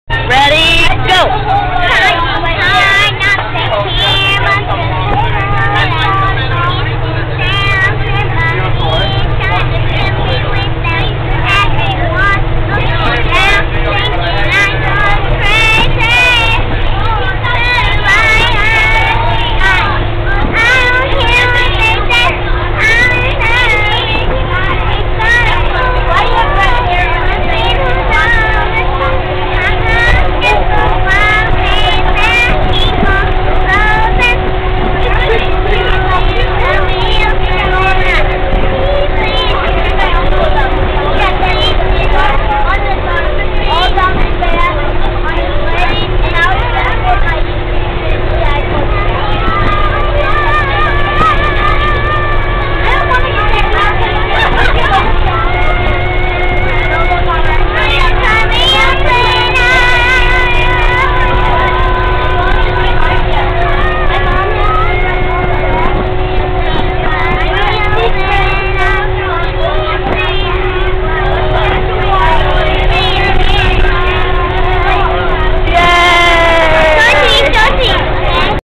Why I love Boys and Girls Club
singin on the bus